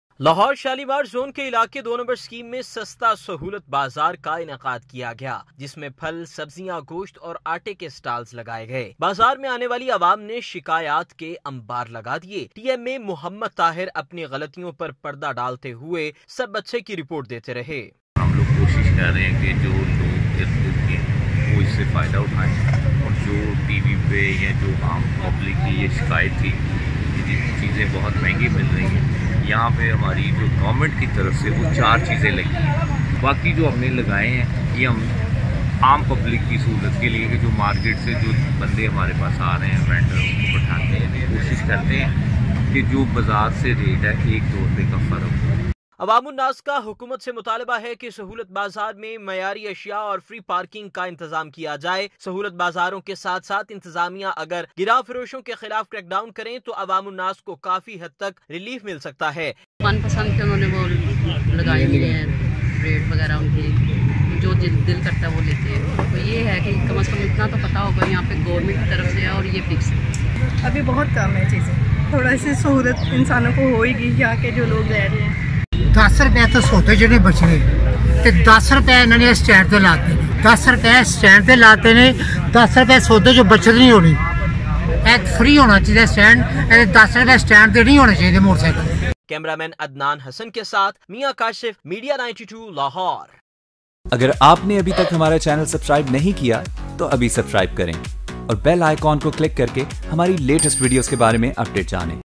Shalimar Zone Mein Sahulat Bazaar | Awaam Ne Shikayat ke Anbaar Laga Diye | M92tv
Cheap bazaar held in Lahore Shalimar zone --- People coming to the bazaar have piled up complaints.

A cheap convenience bazaar was organized in Scheme No. 2 of Lahore Shalimar Zone in which stalls of fruits, vegetables, meat and flour were set up.
The people who came to the bazaar piled up complaints
TMA Muhammad Tahir kept reporting all the good while covering up his mistakes.
The people demanded the government to provide quality goods and free parking in the convenience market.
The public can get a lot of relief if the administration cracks down on the big sellers along with the convenience markets.